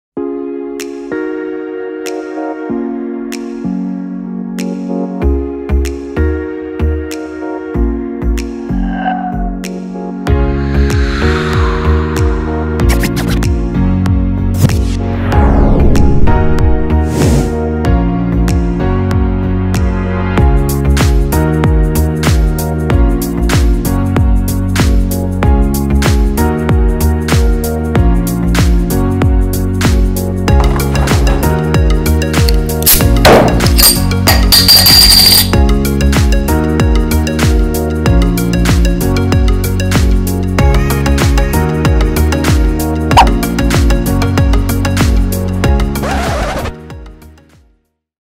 Painting vs Pizza / Edgar Wright Style
As a fan of Peter McKinnon, I decided to challenge myself by creating a video with some of the techniques Peter and Edgar Wright use while making their videos.  It's definitely too long for the challenge, but it is my first try after all.